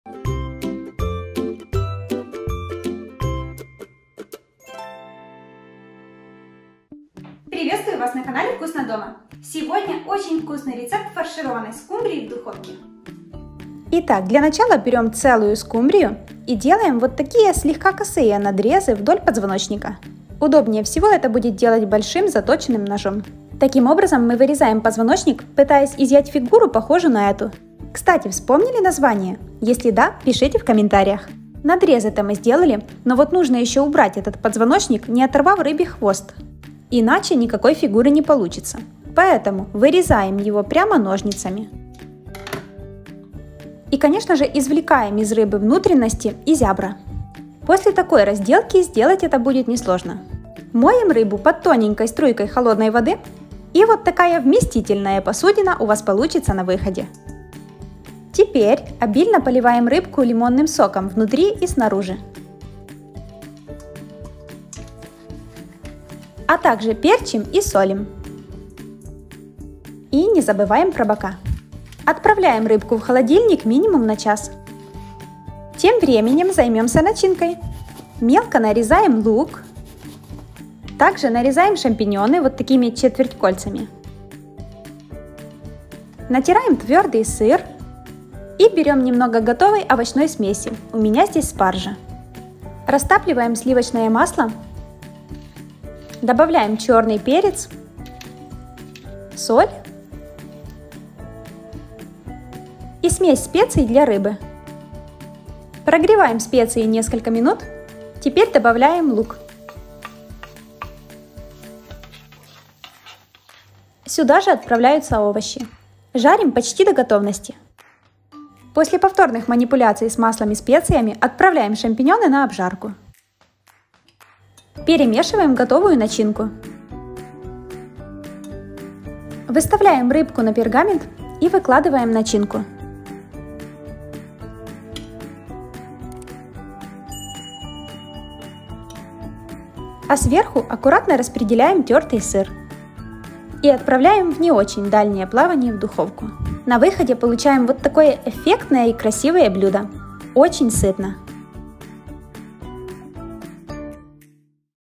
ВКУСНО И КРАСИВО! Фаршированная скумбрия в духовке рецепт
В этом видео вкусный рецепт фаршированной скумбрии в духовке🤗. Блюдо выглядит празднично и эффектно, а по вкусу - находка для любителей фаршированной рыбки!

Ингредиенты для фаршированной скумбрии в духовке:

- скумбрия (2 шт.);
- лимонный сок (с половинки лимона);
- твердый сыр (140 г);
- сливочное масло (50 г);
-  замороженная овощная смесь (200 г).

Выпекать при температуре 180 градусов на протяжении 30 минут.